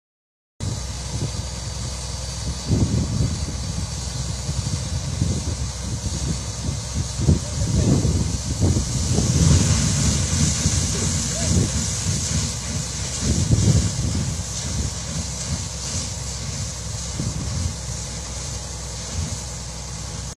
Forage d'eau TUNISIE - Développement Compresseur ROUHIA
Le moment le plus fort 💪
Développement par Compresseur SULLAIR 20_BAR au Forage CHRARDIDA, Rouhia à Seliana en présence des citoyens de la localité 💧
Février 2020

Opérateur:
Société TUNISIE REGENERATION S.A
Immeuble Regency, Bureau 205 les berges du Lac